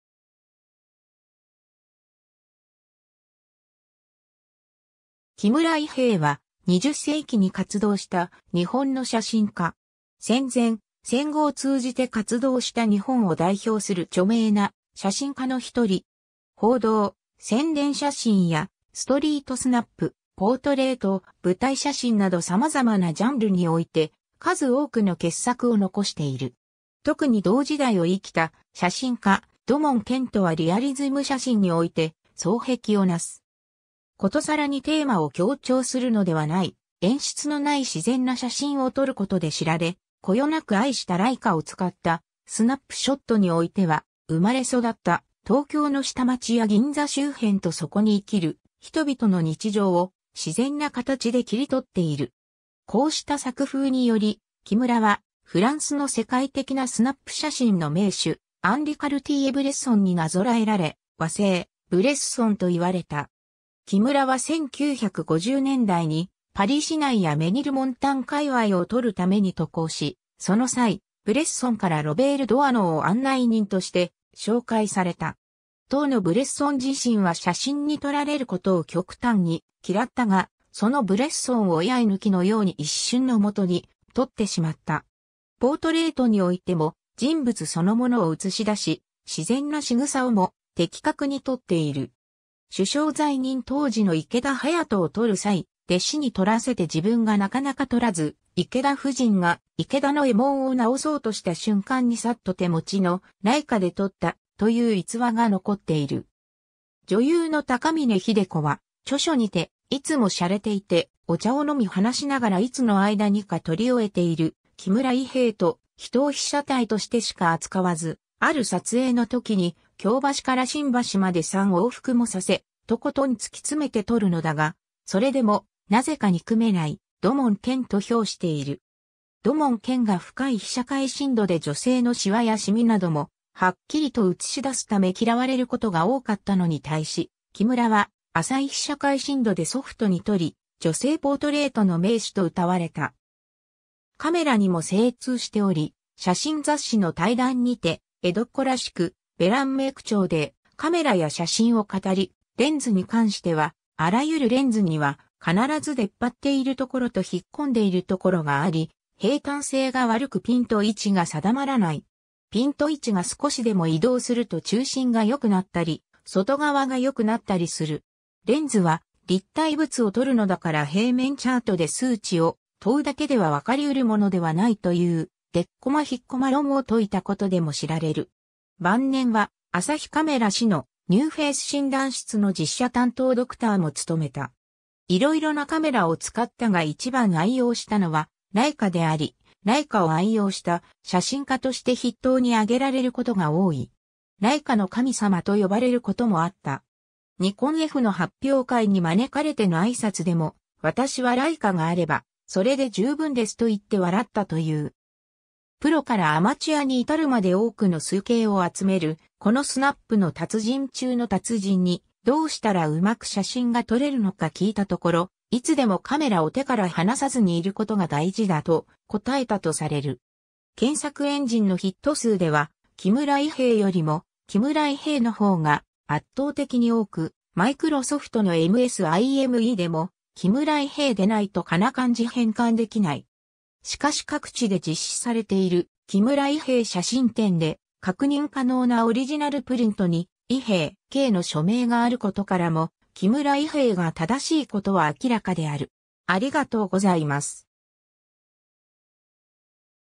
木村伊兵衛
ストリート・フォトグラファー
木村 伊兵衛（きむら いへい、1901年12月12日 - 1974年5月31日）は、20世紀に活動した日本の写真家。
戦前・戦後を通じて活動した日本を代表する著名な写真家の一人。
 報道・宣伝写真やストリートスナップ、ポートレート、舞台写真などさまざまなジャンルにおいて数多くの傑作を残している。
特に同時代を生きた写真家、土門拳とはリアリズム写真において双璧をなす。
 ことさらにテーマを強調するのではない、演出のない自然な写真を撮ることで知られ、こよなく愛したライカを使ったスナップショットにおいては、生まれ育った東京の下町や銀座周辺とそこに生きる人々の日常を、自然な形で切り取っている。
こうした作風により、木村はフランスの世界的なスナップ写真の名手・アンリ・カルティエ＝ブレッソンになぞらえられ"和製ブレッソン"と言われた。
 木村は1950年代にパリ市内やメニルモンタン界隈を撮るために渡航し、その際、ブレッソンからロベール・ドアノーを案内人として紹介された。
当のブレッソン自身は写真に撮られることを極端に嫌ったが、そのブレッソンを居合い抜きのように一瞬のもとに撮ってしまった。
 ポートレートにおいても、人物そのものを映し出し、自然なしぐさをも的確に撮っている。
首相在任当時の池田勇人を撮る際、弟子に撮らせて自分がなかなか撮らず、池田夫人が池田の衣紋を直そうとした瞬間にさっと手持ちのライカで撮った、という逸話が残っている。
 女優の高峰秀子は著書にて、「いつも洒落ていて、お茶を飲み話しながらいつの間にか撮り終えている木村伊兵衛と、人を被写体としてしか扱わず、ある撮影の時に京橋から新橋まで3往復もさせ、とことん突き詰めて撮るのだが、それでも何故か憎めない土門拳」と評している。
 土門拳が深い被写界深度で女性のシワやシミなどもはっきりと写し出すため嫌われることが多かったのに対し、木村は浅い被写界深度でソフトに撮り、女性ポートレートの名手とうたわれた。
 カメラにも精通しており、写真雑誌の対談にて江戸っ子らしくベランメェ口調でカメラや写真を語り、レンズに関しては「あらゆるレンズには必ず出っぱっているところと引っ込んでいるところがあり、平坦性が悪くピント位置が定まらない。ピント位置が少しでも移動すると中心が良くなったり、外側が良くなったりする。レンズは立体物を撮るのだから平面チャートで数値を問うだけではわかり得るものではない」という「デッコマ・ヒッコマ論」を説いたことでも知られる。
晩年は「アサヒカメラ」誌（朝日新聞社）の「ニューフェース診断室」の実写担当ドクターも務めた。
 色々なカメラを使ったが一番愛用したのはライカであり、ライカを愛用した写真家として筆頭に上げられることが多い。
「ライカの神様」と呼ばれることもあった。
ニコンFの発表会に招かれての挨拶でも「私はライカがあればそれで充分です」と言って笑ったという。
 プロからアマチュアに至るまで多くの崇敬を集めるこのスナップの達人中の達人に、どうしたらうまく写真が撮れるのか聞いたところ、『いつでもカメラを手から離さずにいる事が大事だ』と答えたとされる。
 検索エンジンのヒット数では、「きむらいへい」よりも「きむらいへえ」の方が圧倒的に多く、マイクロソフトのMS-IMEでも「きむらいへえ」でないと仮名漢字変換できない。
しかし各地で実施されている木村伊兵衛写真展で確認可能なオリジナルプリントに「Ihei.K」の署名があることからも「きむらいへい」が正しいことは明らかである。